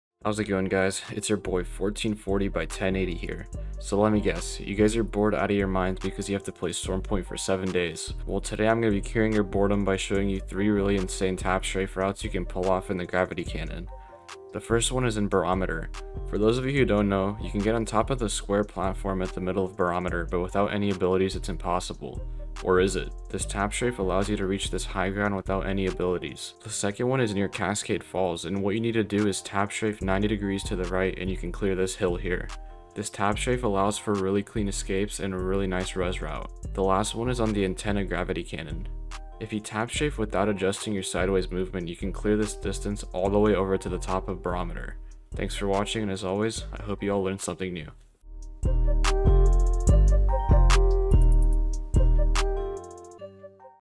3 OP routes for gravity cannon mastery
If you think you have mastered gravity cannon tap strafing, try implementing these routes to make your gameplay more creative and interesting :)